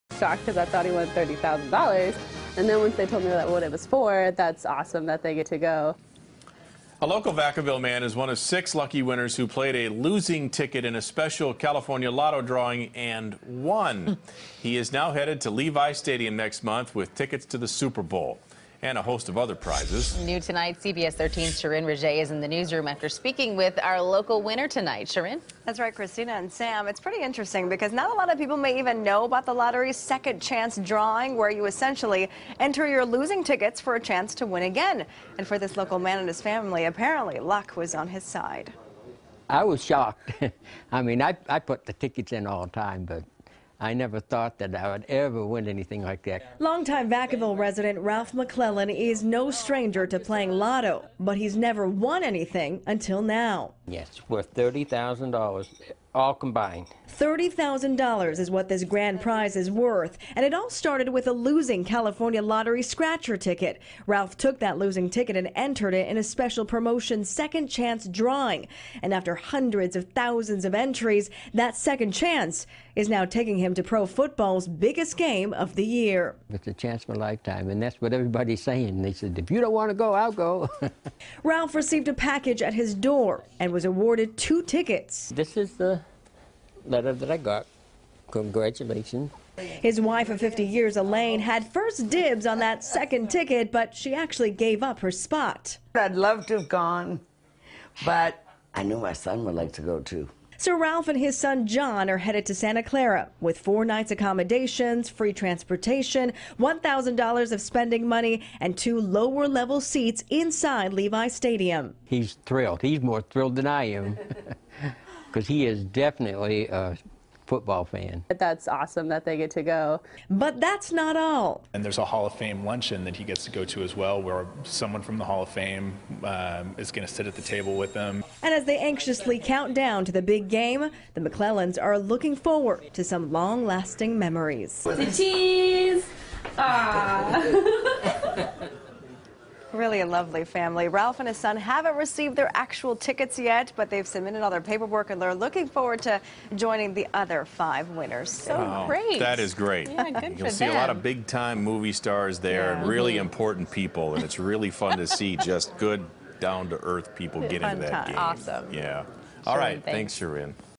Vacaville Man's Losing Lottery Scratcher Wins Him Trip To Super Bowl
Ralph McClellan is no stranger to playing the lottery, but he’s never won anything until now. The road to the $30,000 grand prize started with a losing California Lottery scratcher ticket.